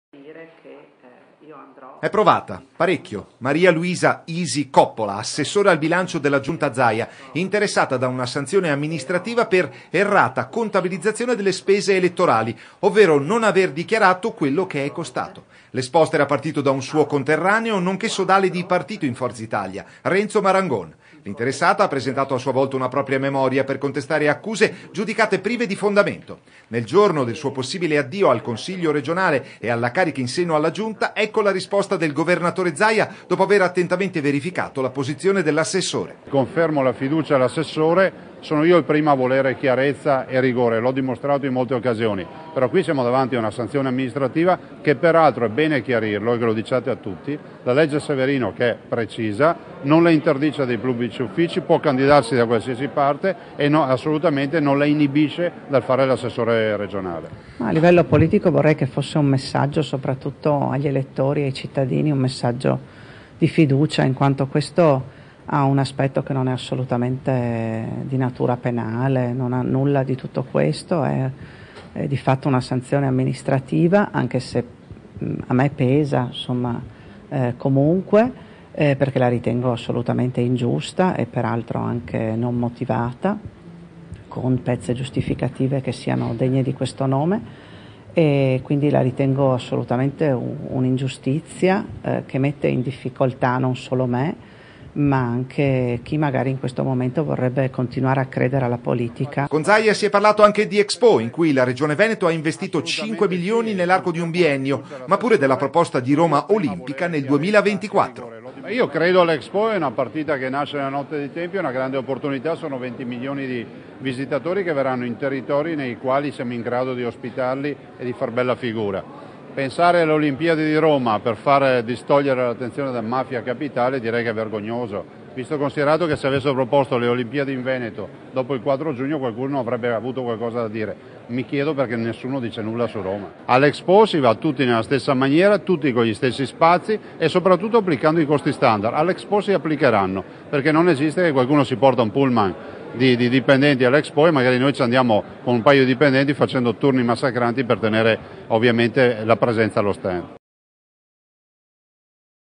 RESTI AL SUO POSTO
Isi Coppola resta al suo posto. L’assessore al Bilancio della Regione Veneto, raggiunta da una sanzione amministrativa con l’accusa di aver “gonfiato” le spese elettorali, non è stata sollevata dall’incarico. A deciderlo, il Governatore Luca Zaia in persona.